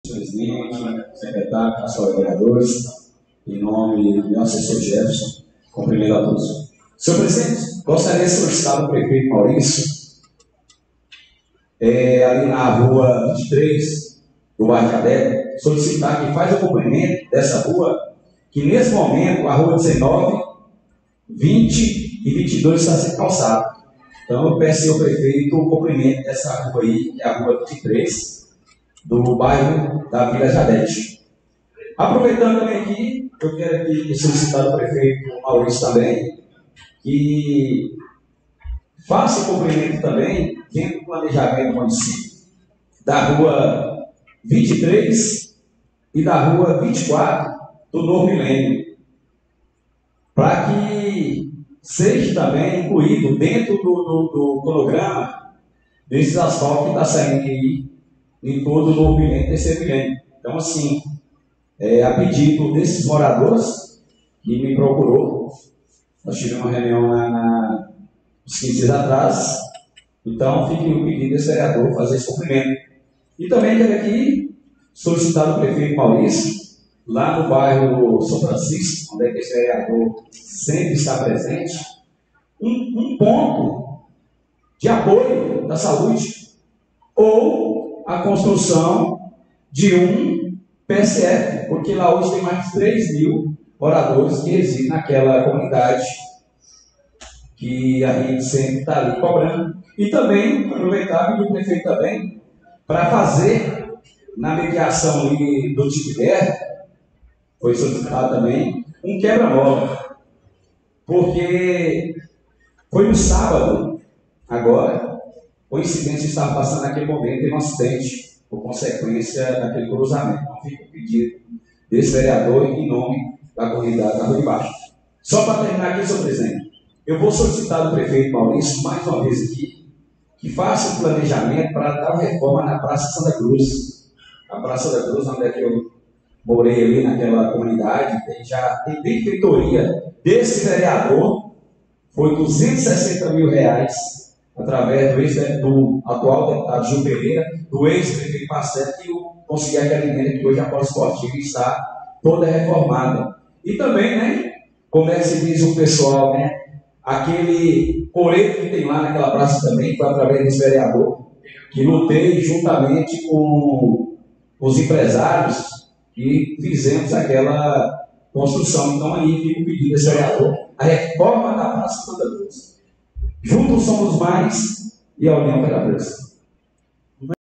FALA EDIL LUIZ PEQUI (AGIR) - 12ª REUNIÃO - TEMA: PONTO DE APOIO DA SAÚDE NO BAIRRO SÃO FRANCISCO;
WEBTV CORREIO DO VALE DO SÃO FRANCISCO
PROGRAMA CIDADE LIVRE - 28/08/23
PRONUNCIAMENTO DO VEREADOR LUIZ PEQUI (AGIR) - 12ª REUNIÃO ORDINÁRIA DA CÂMARA MUNICIPAL DE JANUÁRIA - 19:00h - 21/08/23. TEMA: PONTO DE APOIO  DA SAÚDE NO BAIRRO SÃO FRANCISCO E OUTRAS QUESTÕES;
FAMILIA - FÉ - DEMOCRACIA SEMPRE. LUTAMOS PELA LIBERDADE DE EXPRESSÃO E CONTRA A REGULAÇÃO DA MÍDIA.